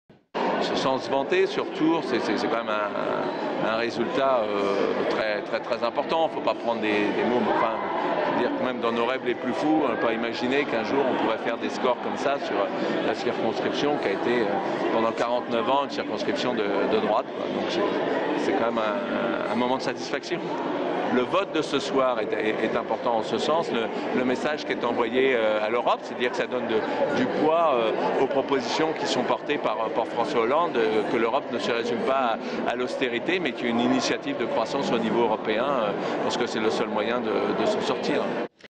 Législatives: réaction de Jean-Patrick Gille
Sites webs/blogs, copiez cette vidéo avec le bouton "Intégrer" !
 Réaction de Jean-Patrick Gille, député PS, 1ère circonscription d'Indre-et-Loire.